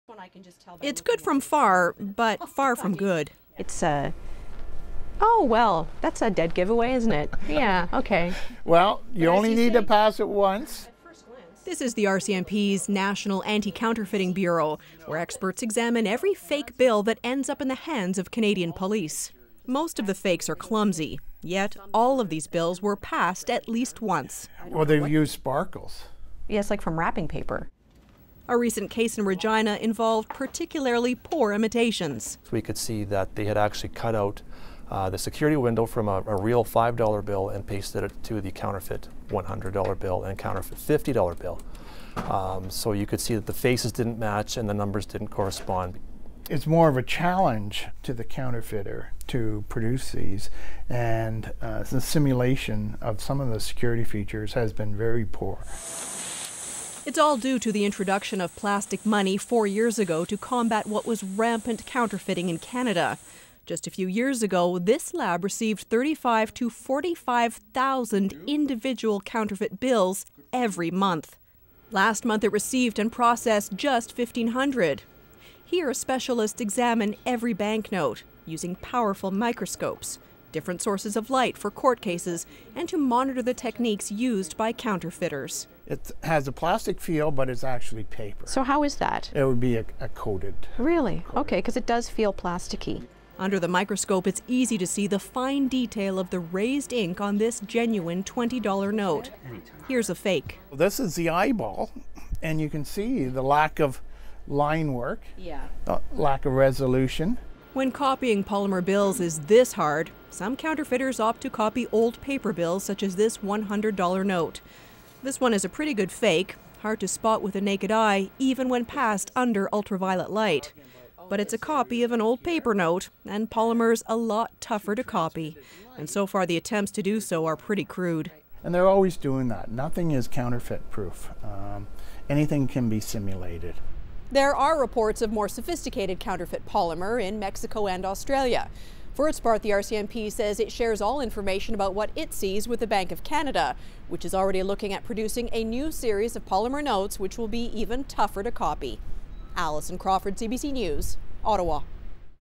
Counterfeiters perplexed by Canada's plastic money
Canada's plastic money is stumping counterfeiters. The RCMP estimate the number of fake bills passed on to retailers in 2015 dropped by 74 per cent compared with the previous year.